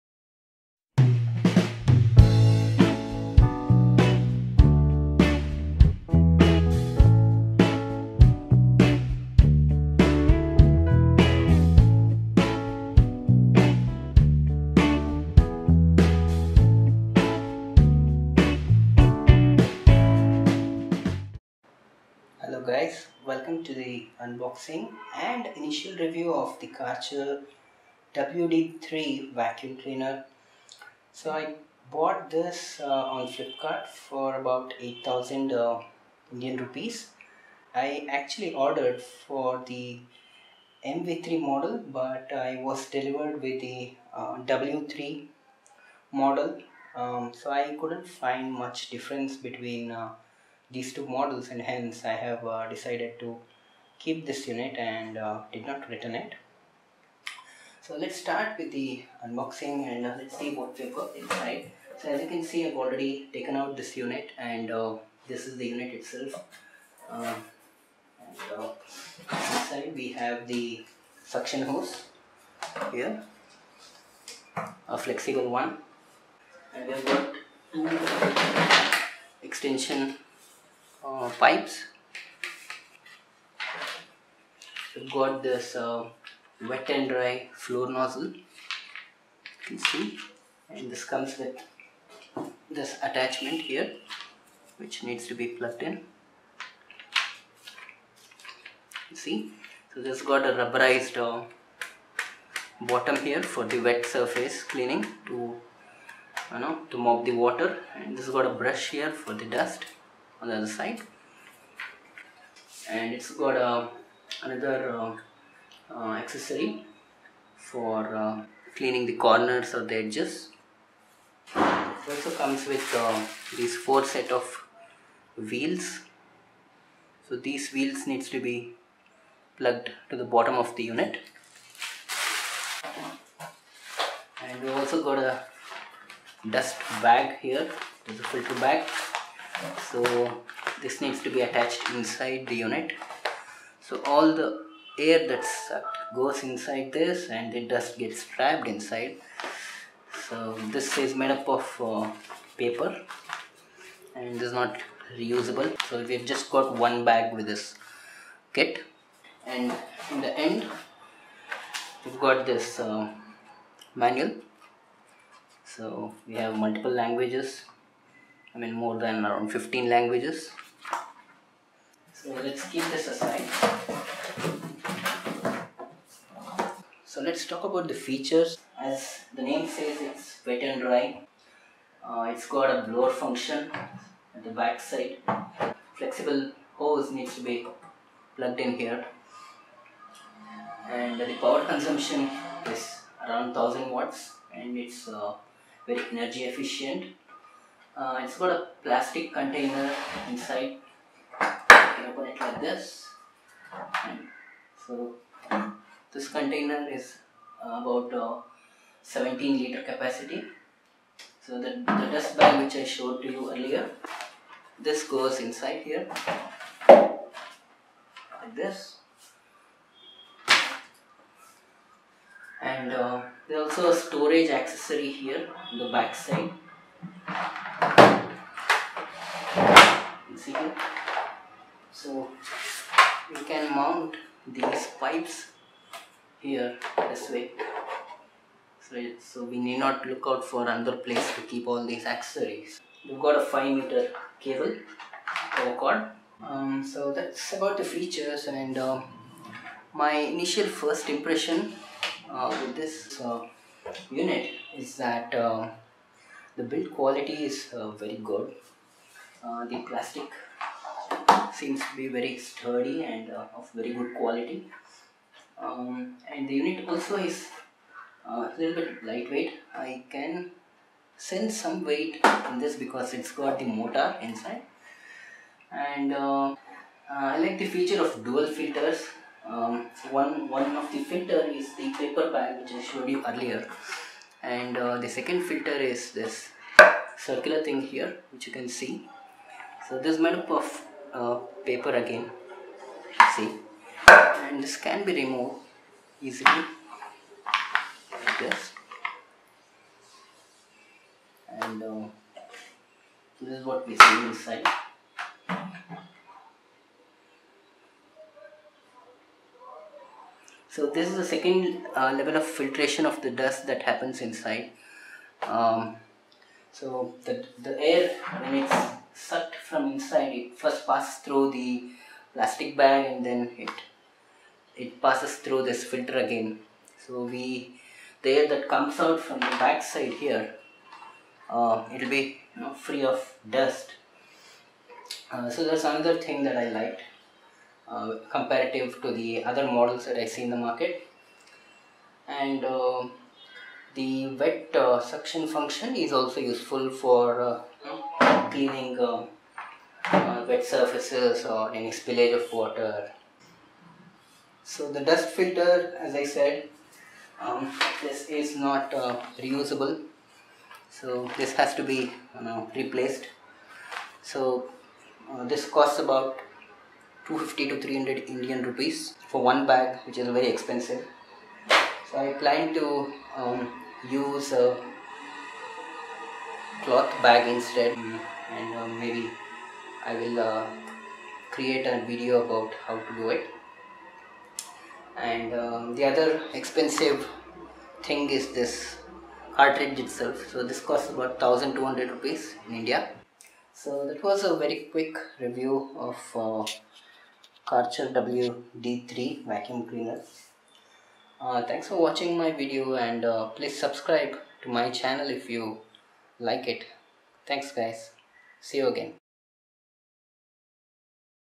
Karcher MV3 / WD3 Vacuum Cleaner - Unboxing & Initial Impressions
Karcher wd3/mv3 1000-watt wet and dry vacuum cleaner

Features:
1. Multipurpose – dry and wet
2. Its got blower function to blow off leaves or dust
3. Power consumption is 1000W which is energy efficient
4. Its got a plastic container with a capacity of 17 litre capacity
5. Storage accessory on back side
6. 5 meter power cable

(Karcher Wd 3)
(Karcher Wd 3 Premium)
(Karcher WD 5)